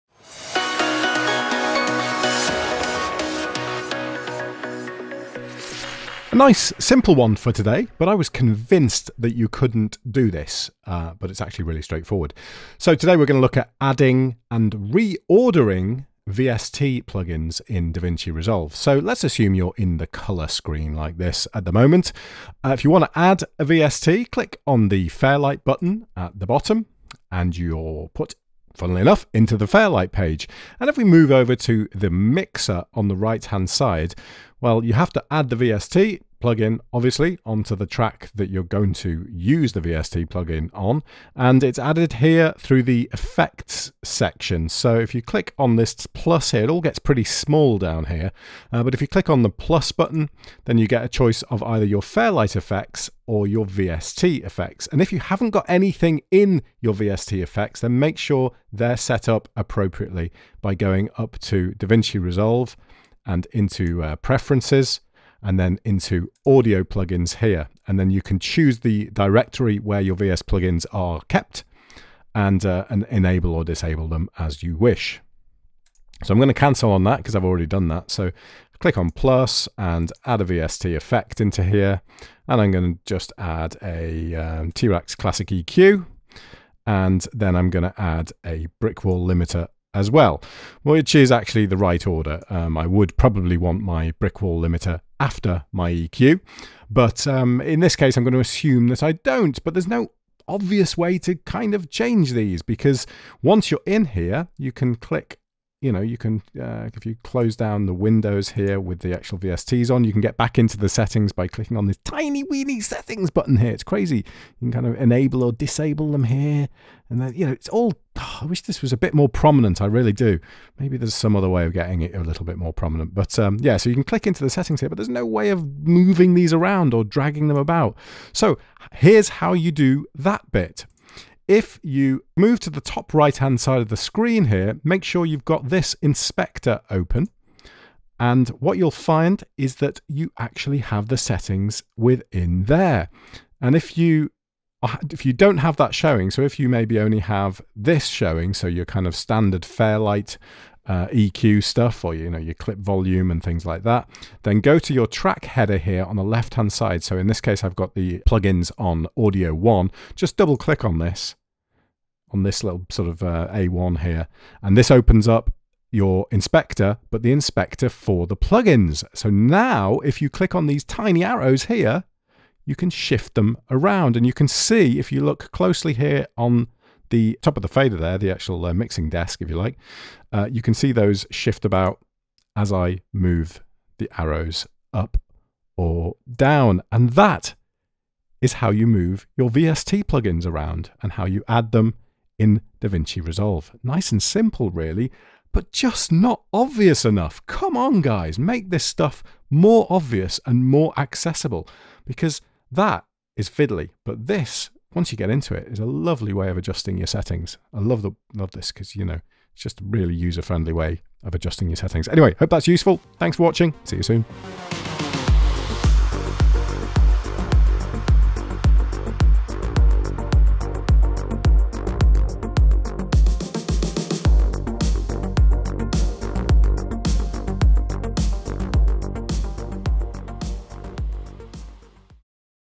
Changing Order & Setting Up VST Plugins | Davinci Resolve Tutorial
A short tutorial on how to change the order, set up and add VST plugins in Davinci Resolve 15.

I was convinced that this couldn't be done because it's really not obvious at all. If you're from a DAW background, the fact that VST and Fairlight FX almost seem of little importance in the software is strange. It's like they've intentionally made this difficult to find. It's not exactly like using Ableton Live.

Anyway, it's easy when you know how so hopefully you'll find this useful.

Footage Location: Bristol, UK
Davinci Resolve Version: Studio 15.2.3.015